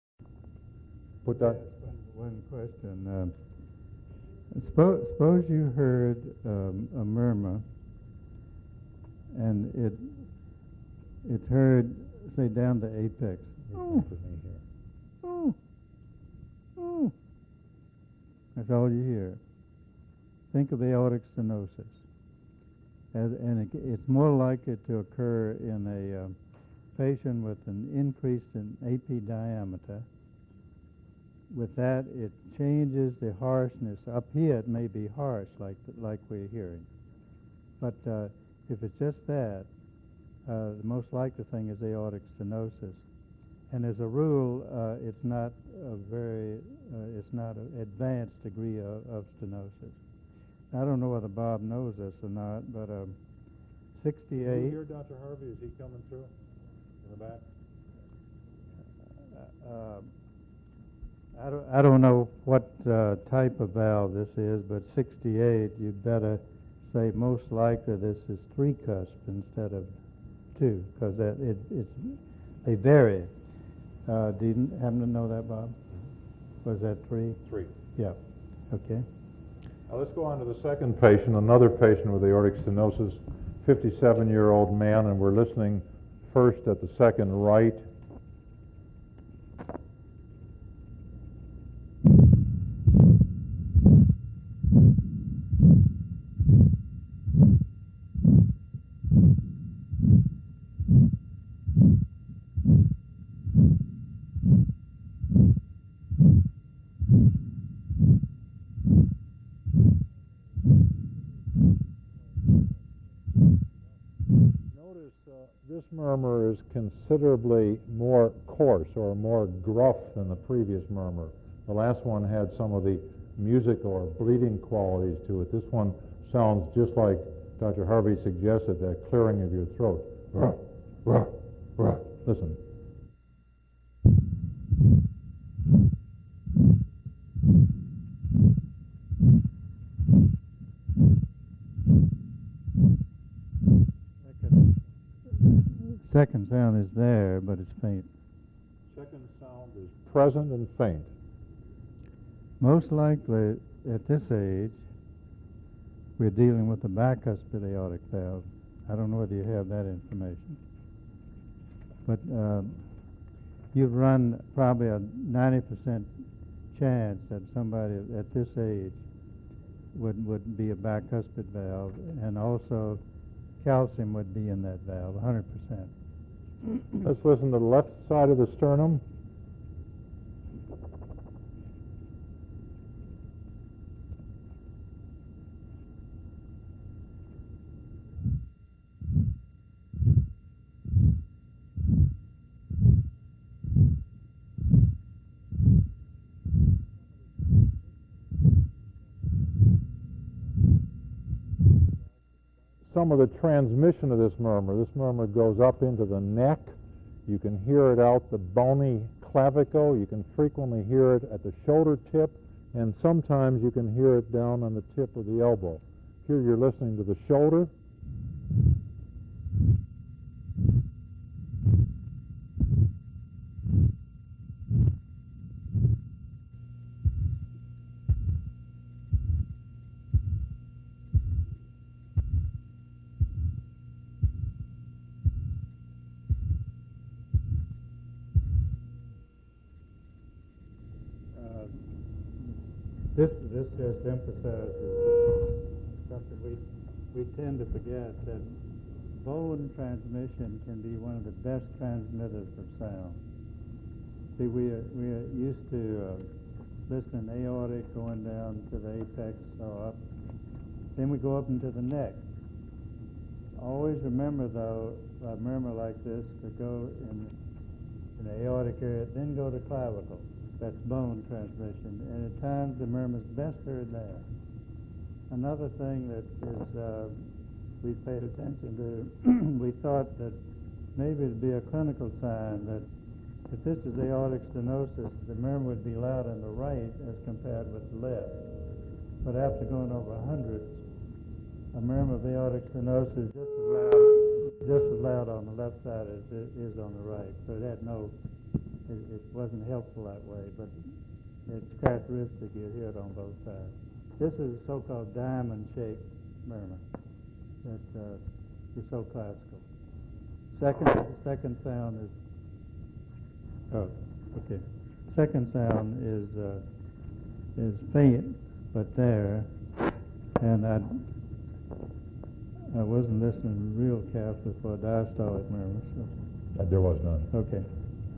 Hall03
Dr. Robert J. Hall presenting at the Texas Heart Institute 1993  Annual Spring Auscultation Symposium. Part 3.